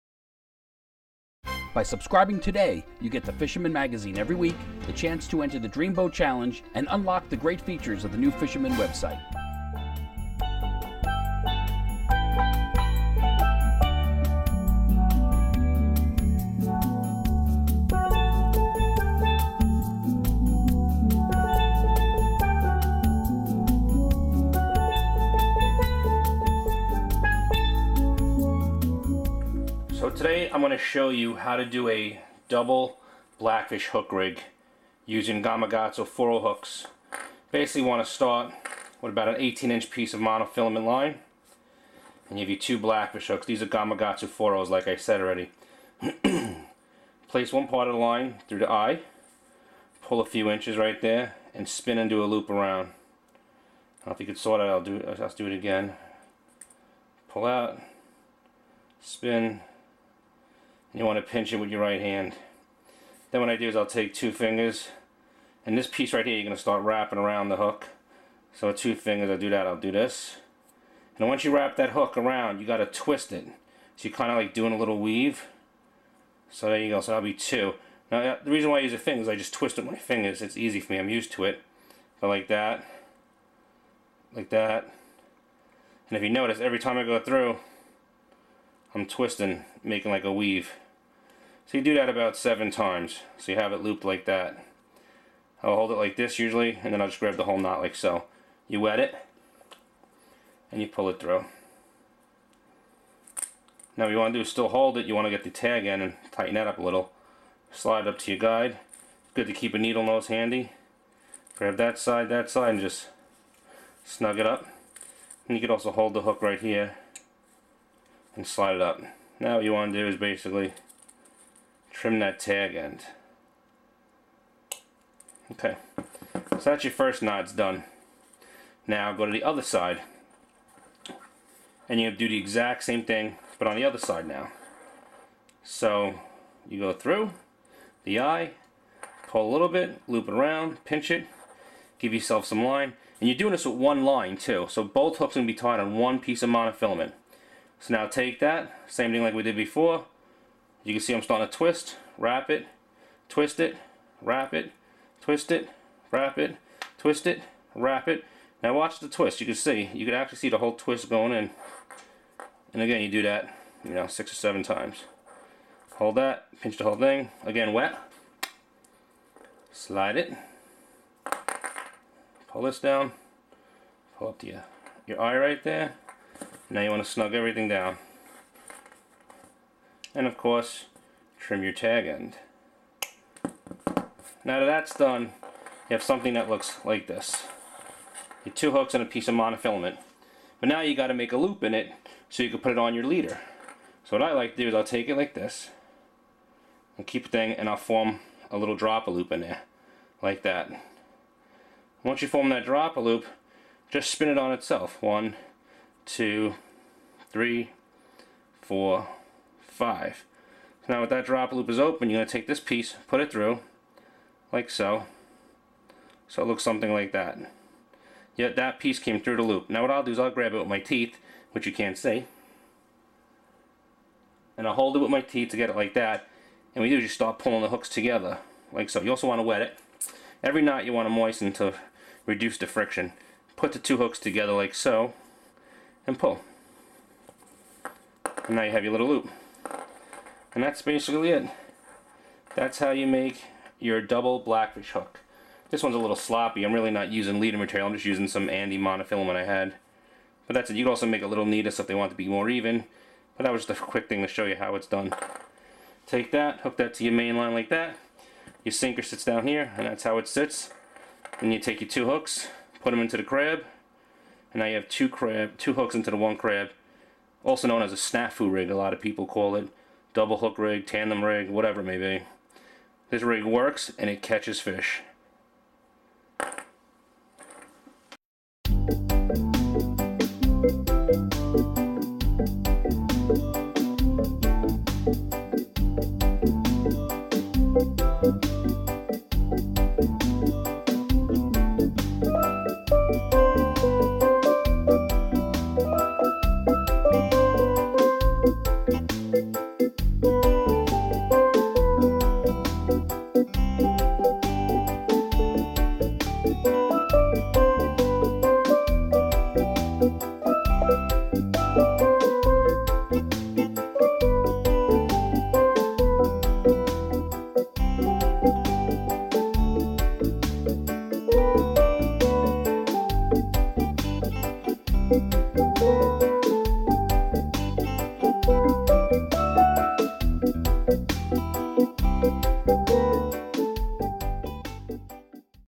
How To Tie a Double Blackfish Rig / Snafu Rig Using One Piece of Leader and Two Hooks
Learn how to tie one of the deadliest rigs to use on Blackfish. The Snafu rig gets them every time. Even when a hook pulls from a fishes mouth the second hook can snag them and you still get your fish. For more great videos register on our page at www . ChasinTailTV .com